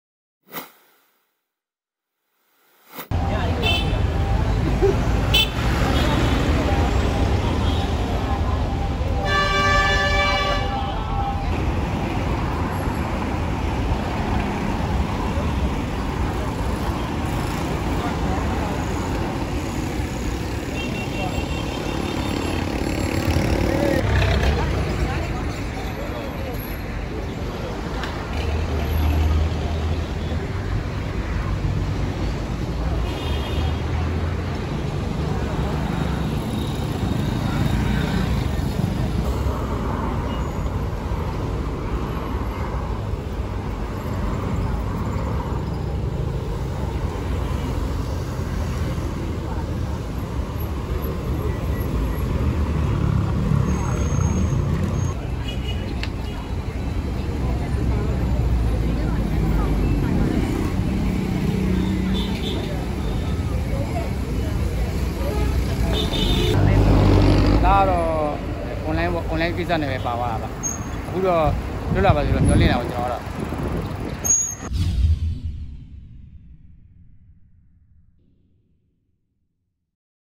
People wait outside Myanmar jail as junta to release prisoners
People wait outside a jail in Yangon as Myanmar's ruling military junta says it will release almost 6,000 prisoners, including a former British ambassador, a Japanese journalist and an Australian adviser to Aung San SuuKyi's ousted government.